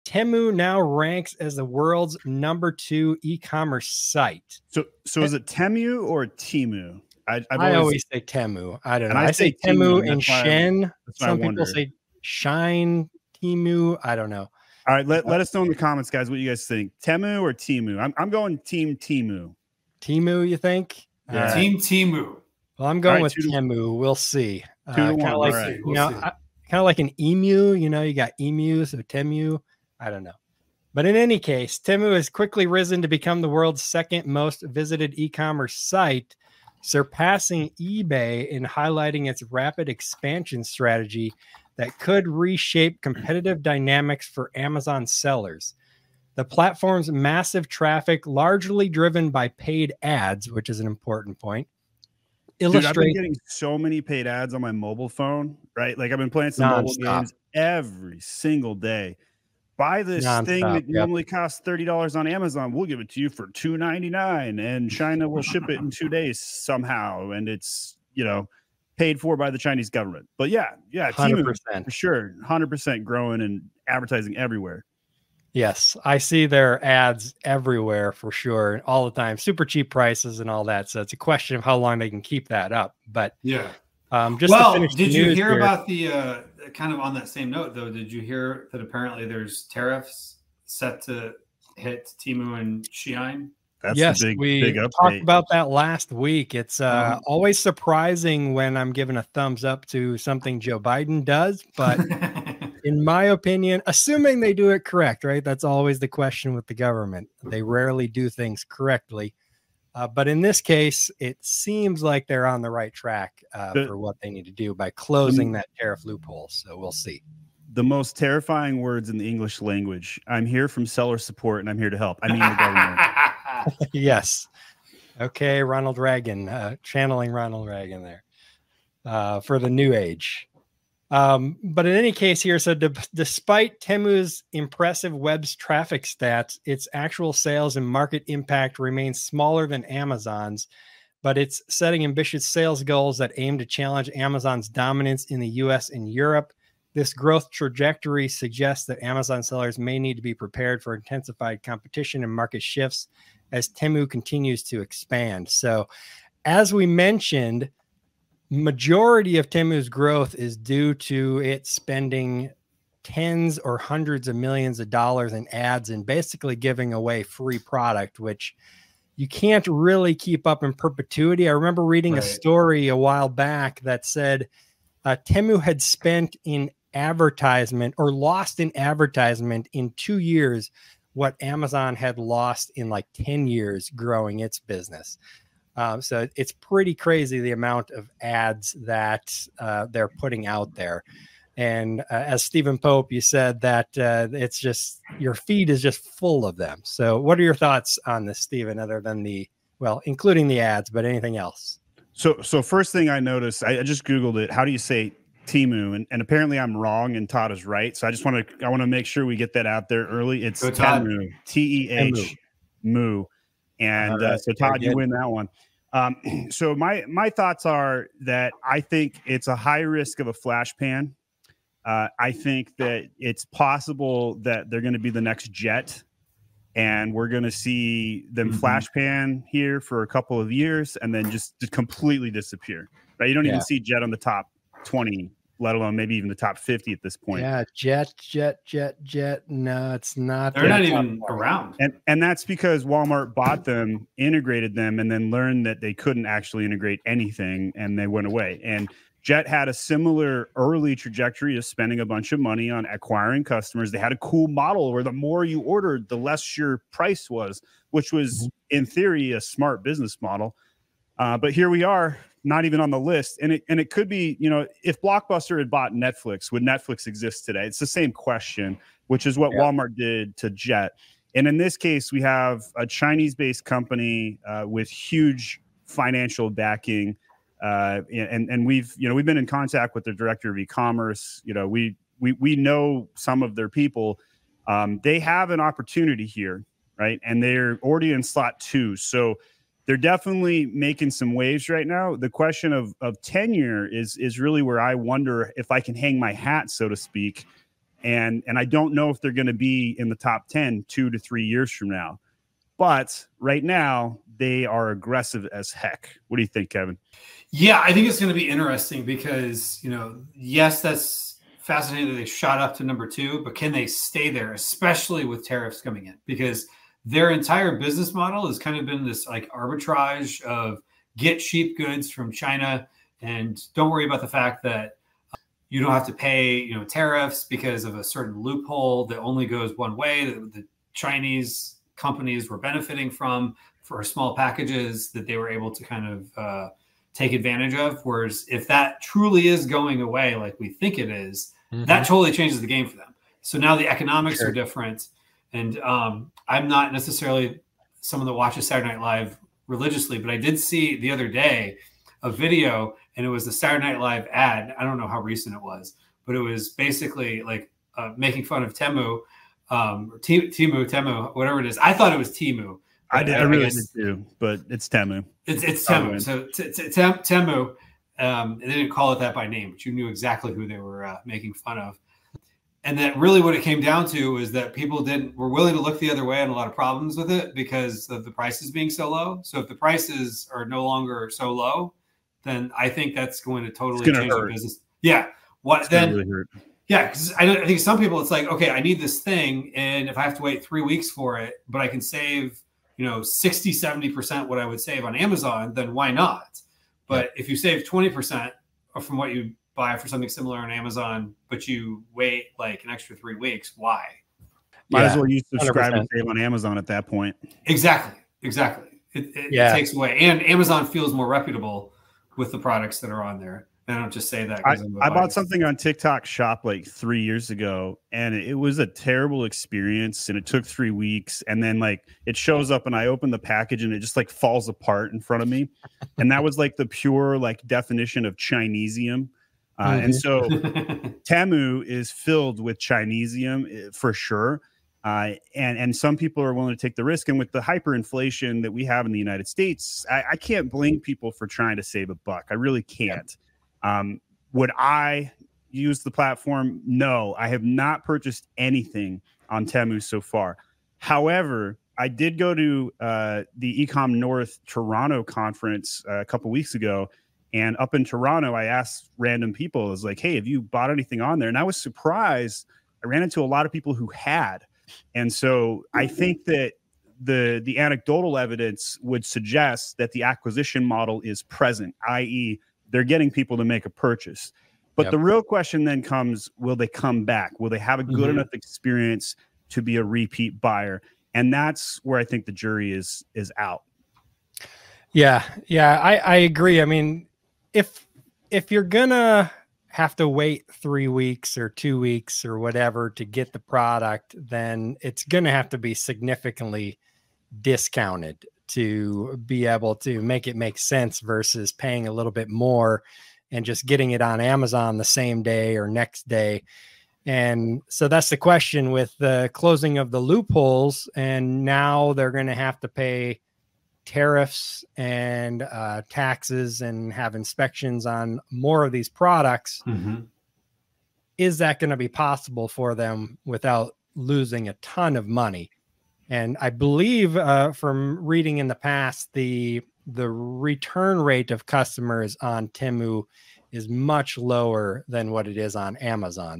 Is Temu the Next Big Threat to Amazon? Exploring the E-Commerce Giant's Rapid Rise
Temu has quickly climbed to the number two spot in the global e-commerce market, surpassing eBay. But can this rapid growth last? In this video, we explore how Temu’s aggressive ad strategy and ultra-low prices have driven its success. We also discuss the impact of upcoming tariffs and whether Temu's business model can withstand these changes. Is Temu here to stay or just a flash in the pan? Join us as we dive into the potential long-term effects on Amazon sellers and the future of the e-commerce landscape.